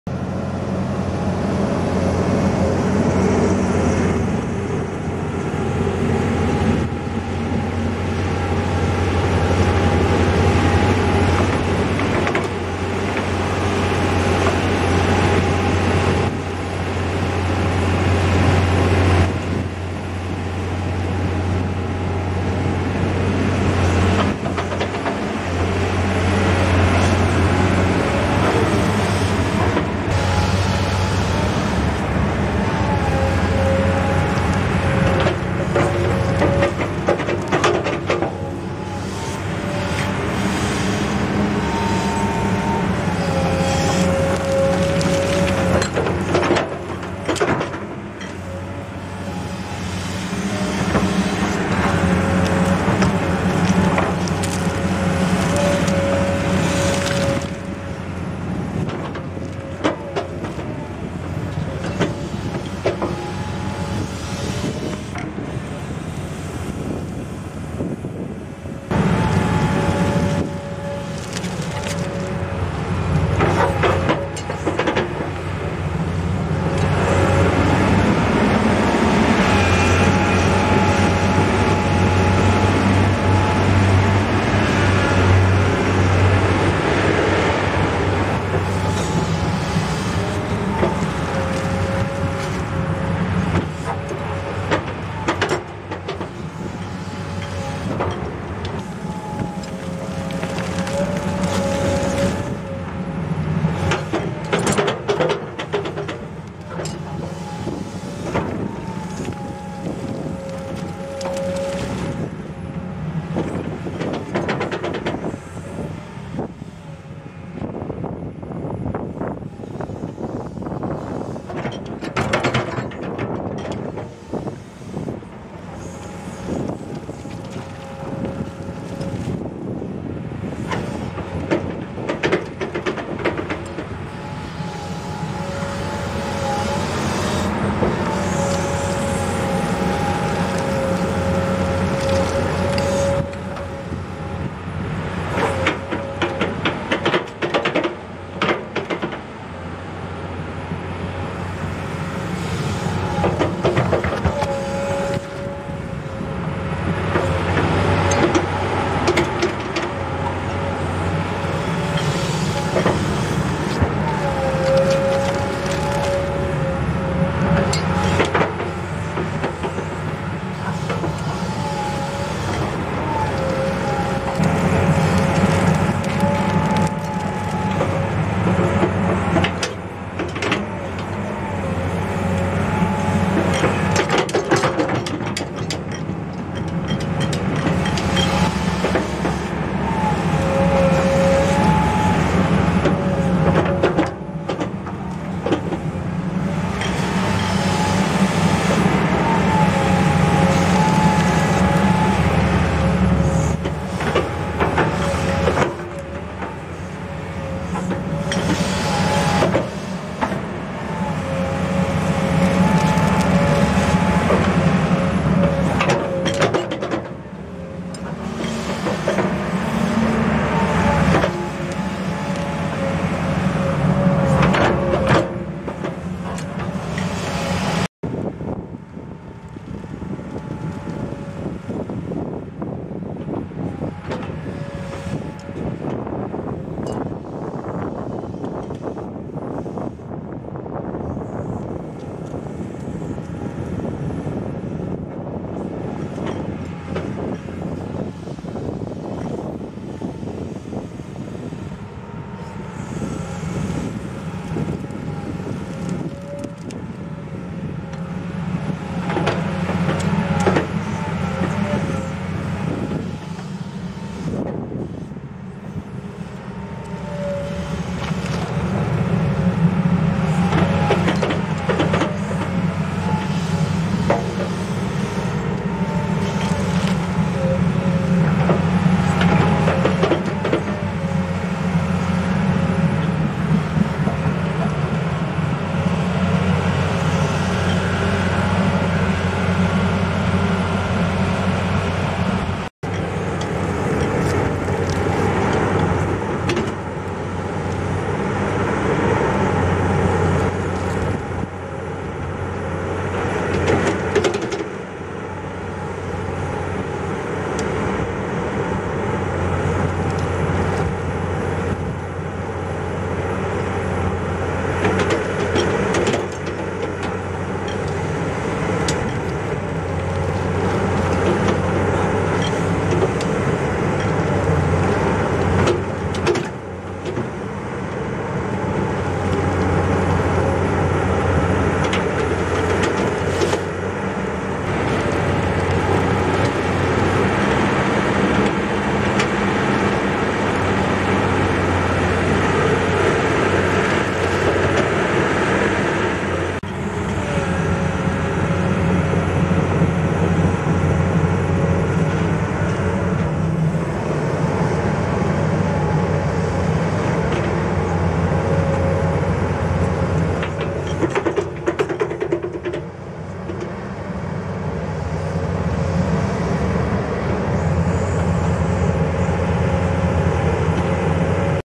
Farm Drainage System | Ditch Digging | Excavator Tilt Bucket
You will see digging farm drainage ditches with an excavator and a sloping bucket in this video. This is a large farm and needed a new water drainage system. New ditches were dug for the farm.